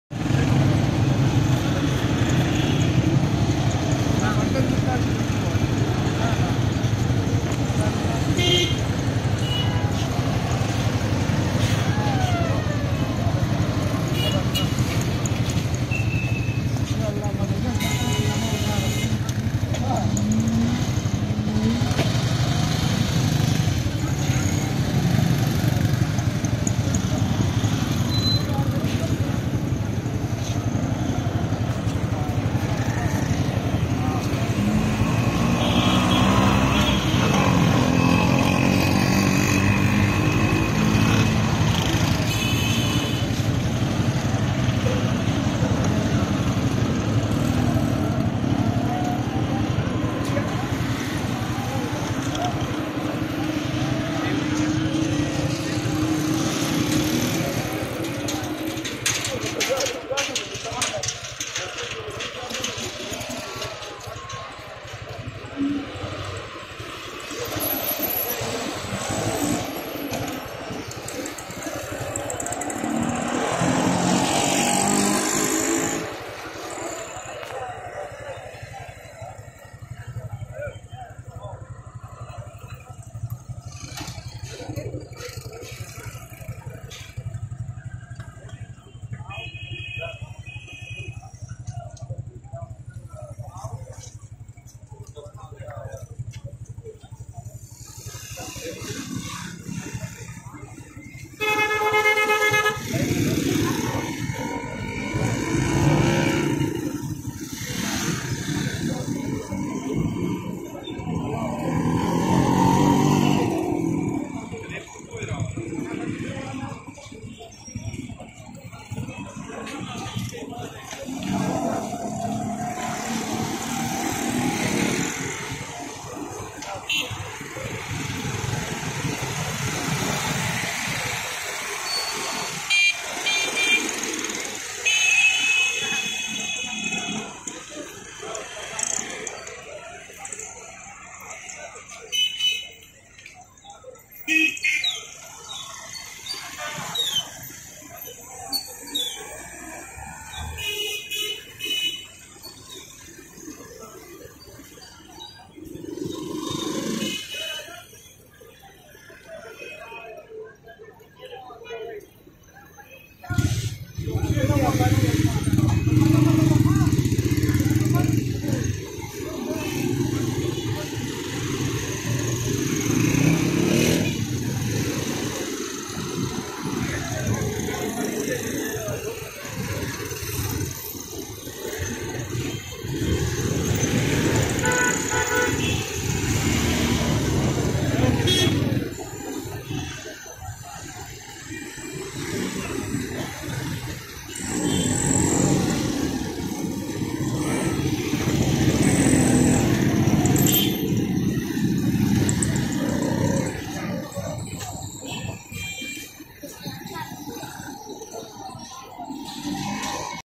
Road intersection in Pakistan
Busy traffic at traffic crossing on the roads
Local Transportation such as tuk-tuk, motorcycles, cars and vans etc.
Daily commerce and travel of local population in their ethnic clothing
Police warden directs traffic